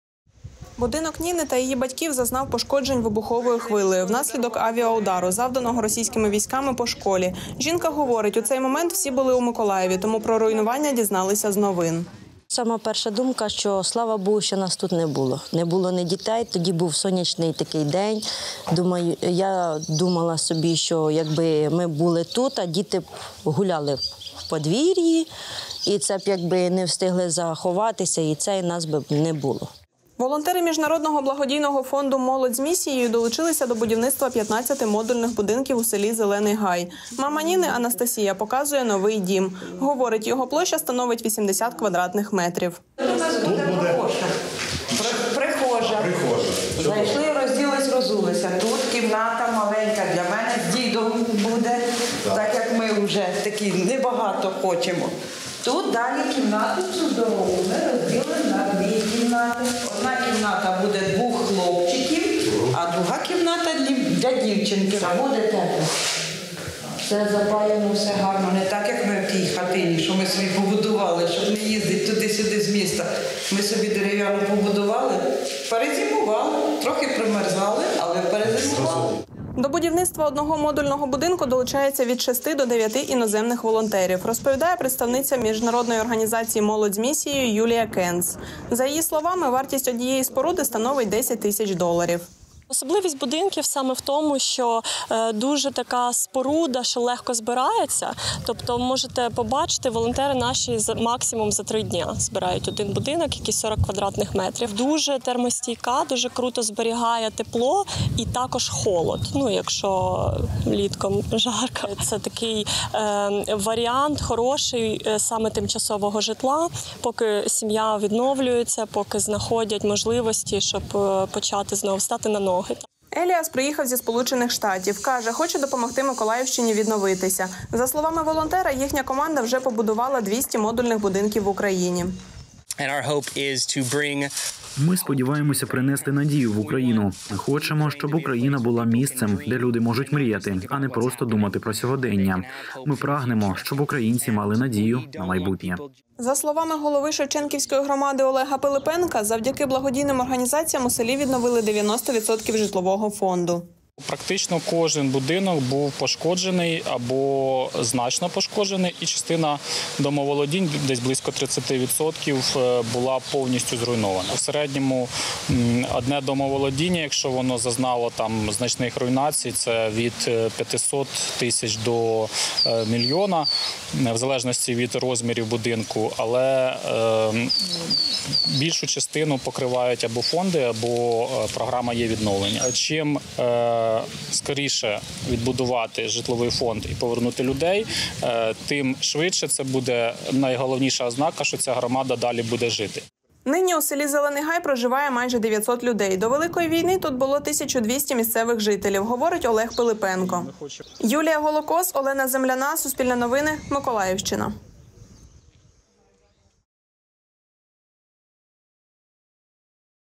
У селі на Миколаївщині встановлюють модульні будинки
На Миколаївщині у селі Зелений Гай волонтери встановлюють 15 модульних будинків. Вони термостійкі, легко збираються і стануть тимчасовим прихистком для людей, які внаслідок російських обстрілів втратили житло.